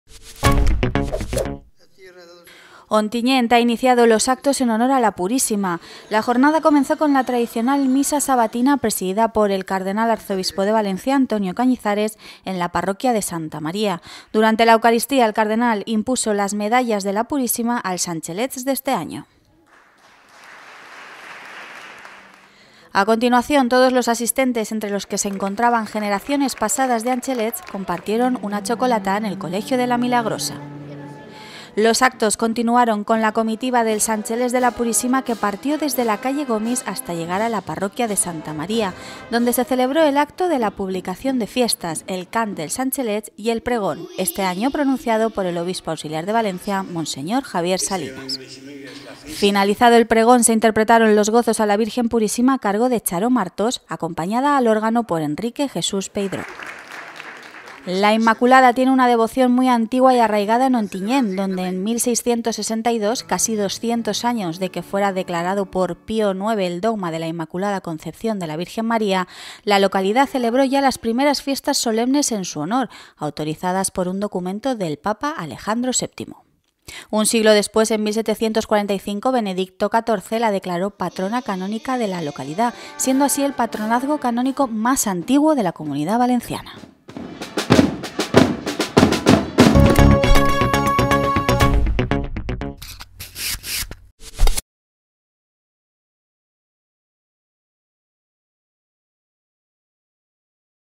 Ontinyent comienza los actos en honor a la Purísima
El cardenal arzobispo de Valencia Antonio Cañizares presidió la tradicional Misa Sabatina en la parroquia de santa María de Ontinyent , tras la que bendijo e impuso las medallas de la Purisíma a los ‘Angelets’ de estas fiestas.

En la misa en honor a la Purísima Concepción participaron “todas aquellas personas que han tenido el honor de ser Angelets de la Puríssima”, según ha confirmado Juan Melchor Seguí, párroco de santa María. Acabada la eucaristía, los asistentes pudieron disfrutar de una “xocolatada” en el colegio La Milagrosa.